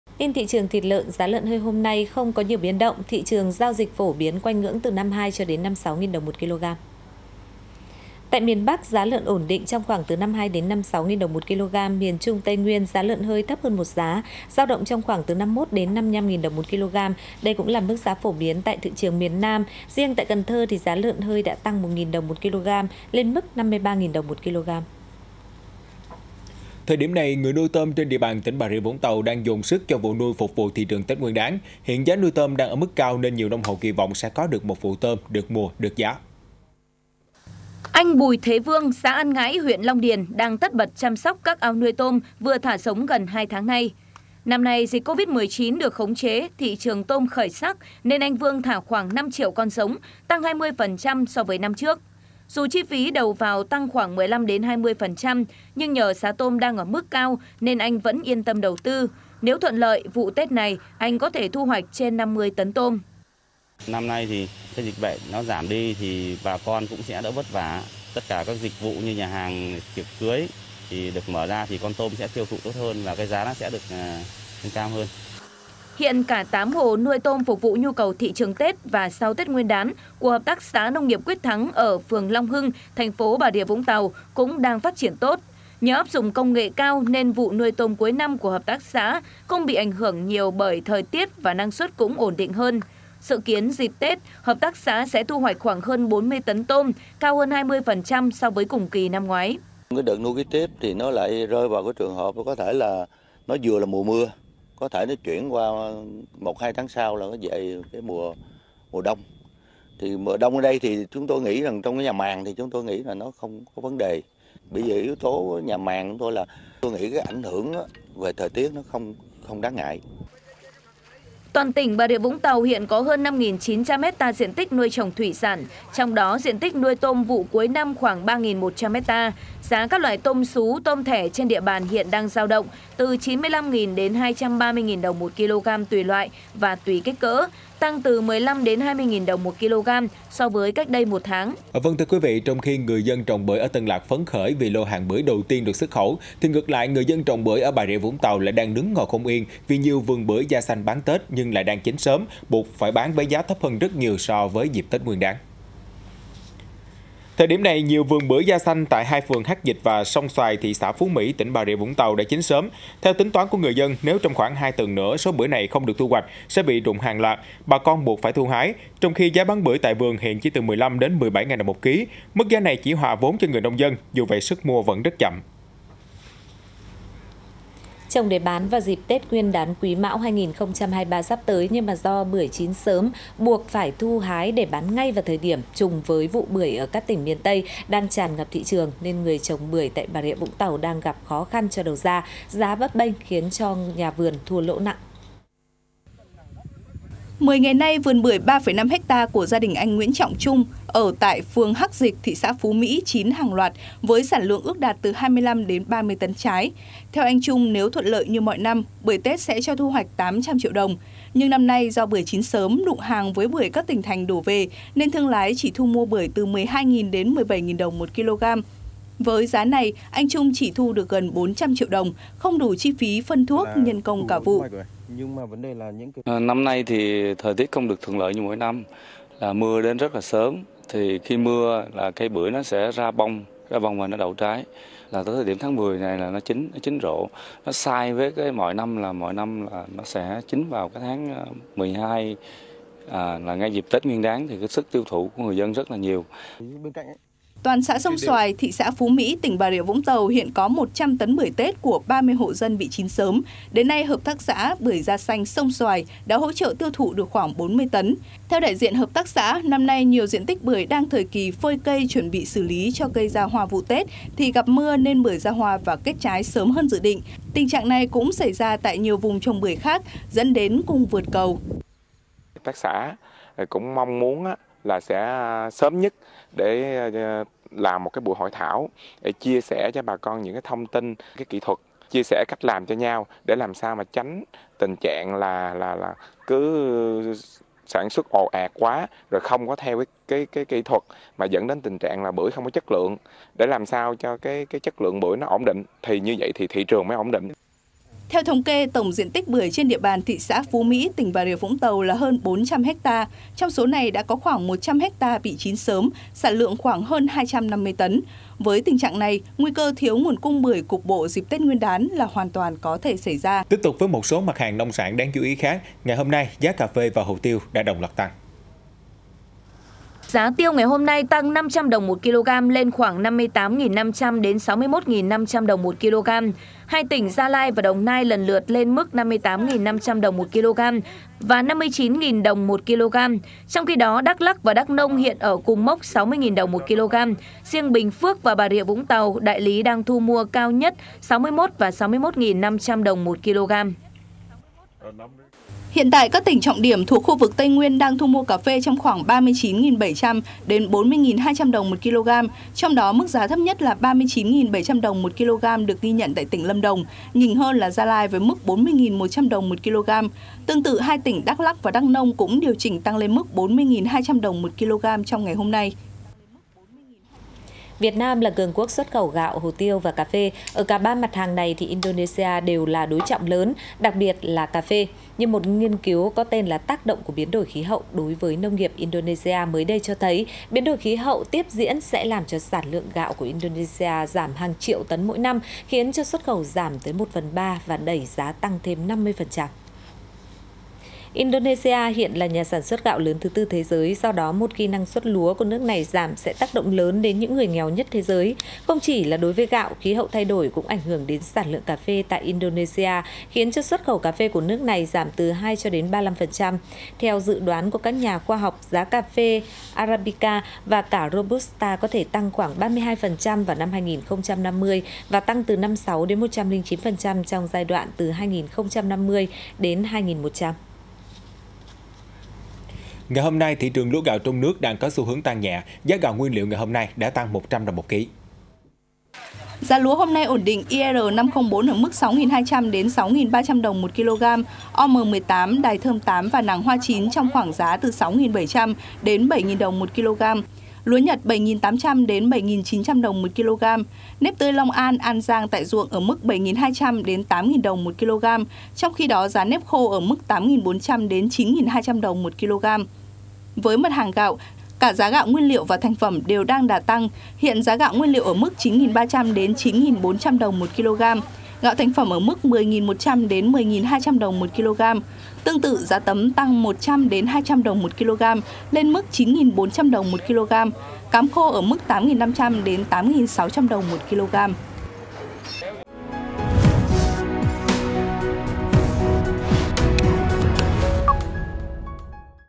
Giá lợn hơi cao nhất 56.000 đồng/kg | VTC16
VTC16 | Giá lợn hơi cao nhất 56.000 đồng/kg | VTC16. Tin thị trường thịt lợn. Giá lợn hơi hôm nay không có nhiều biến động. Thị trường giao dịch phổ biến quanh ngưỡng 52 – 56.000 đồng/ kg.